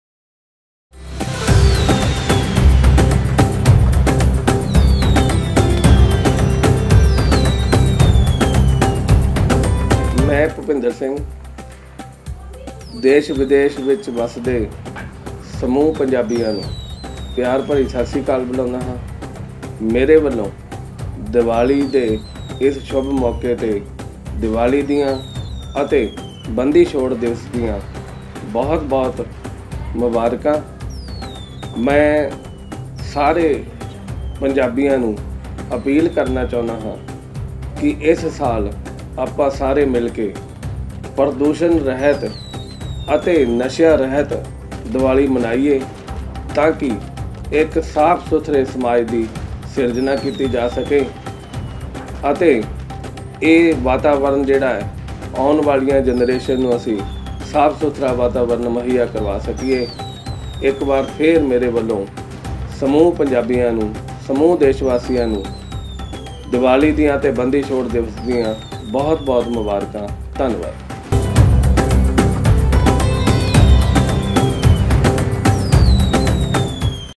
Bhupinder Singh | Wishes Happy Diwali 2018 | ਦੀਵਾਲੀ ਦੀਆਂ ਸ਼ੁਭਕਾਮਨਾਵਾਂ
Bhupinder Singh | Wishes Happy Diwali 2018
Director, Mata Gujri Sr. Sec. School Guthmada

ਦੀਵਾਲੀ ਅਤੇ ਬੰਦੀ ਛੋੜ ਦਿਵਸ ਮੌਕੇ ਸ਼ੁਭਕਾਮਨਾਵਾਂ